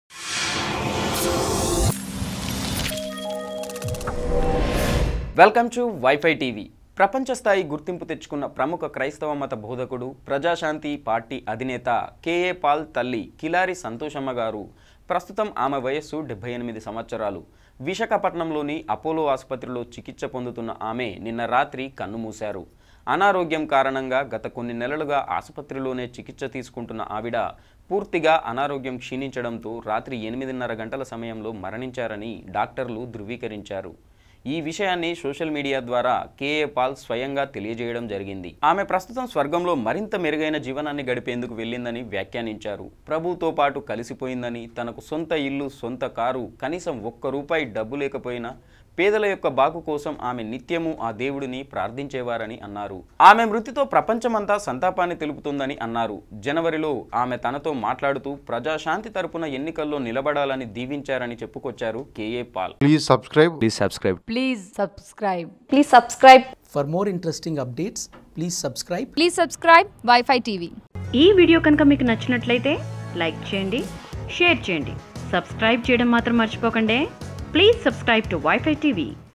KA Paul Mother Santhosamma Passed away | KA Paul Mother No More | Political News | Y5 Tv
Kilari Anand Paul is a Christian evangelist from Southern India. He was living in Houston as of 2003. He runs a ministry called Gospel to the Unreached Millions.
Originally named Kilari Anand Paul, he was born on 25 September 1963, in a small village called Chittivalasa located in the State of Andhra Pradesh,India, into a Hindu family. His parents were Barnabas and Santhosamma
His parents converted to Christianity in 1966. Paul became a Christian in March 1971, when he was eight years old. Paul says he travelled with his evangelist father to hundreds of villages in India sharing the gospel with many non-Christians. Paul says that at the age of 19 he entered into the full-time ministry.